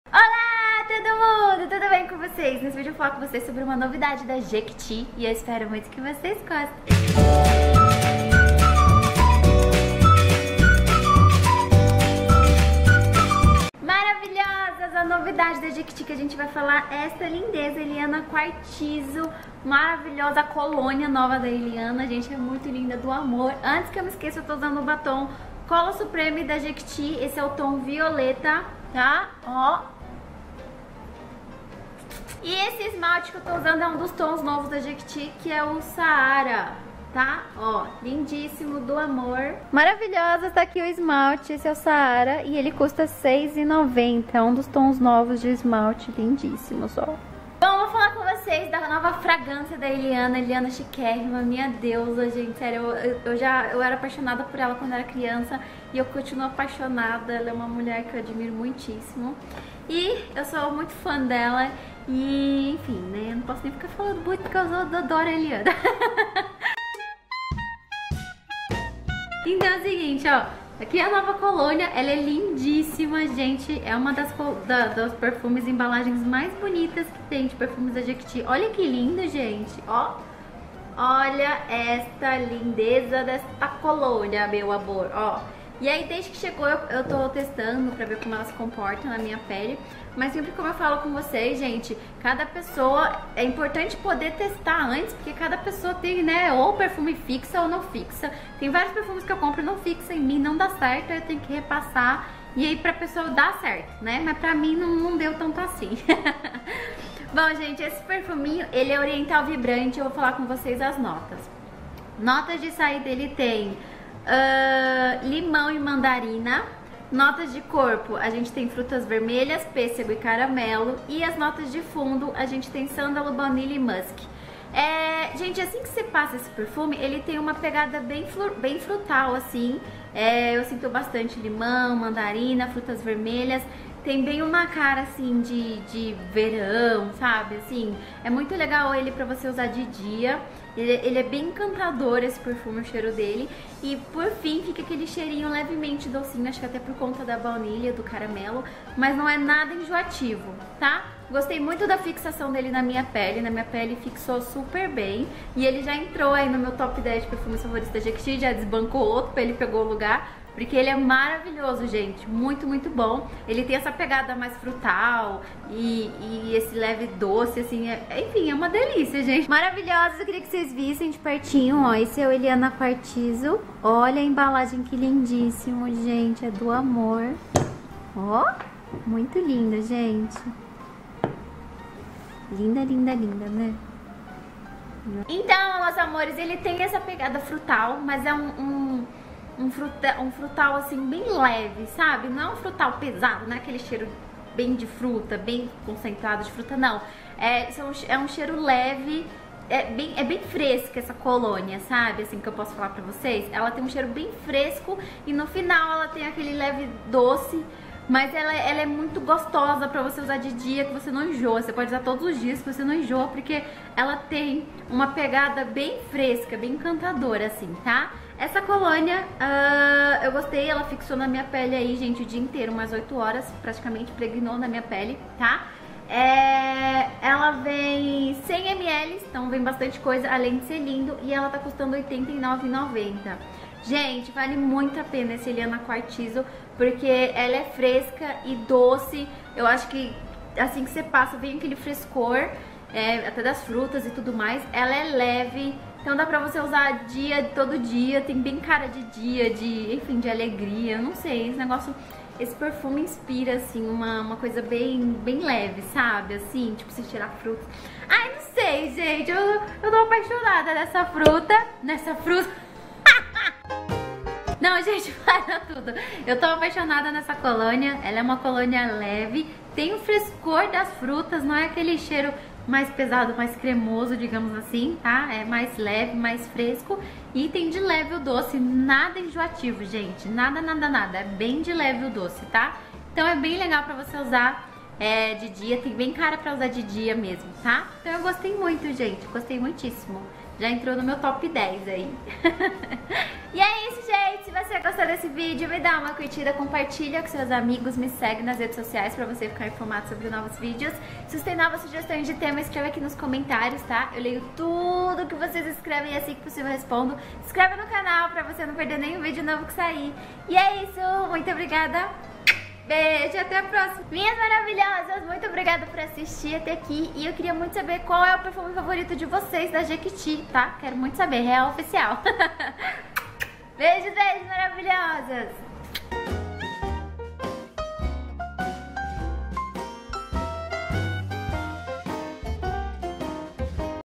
RESENHA: COLÔNIA ELIANA QUARTZO | LAIANE LIRA
Confira a resenha da nova Colônia da Jequiti a Eliana Quartzo.

INSTAGRAM DO CANAL: @laianeliraoficial
INSTAGRAM DO FÃ CLUBE: @fc_laianeliraoficial

Obrigada! :)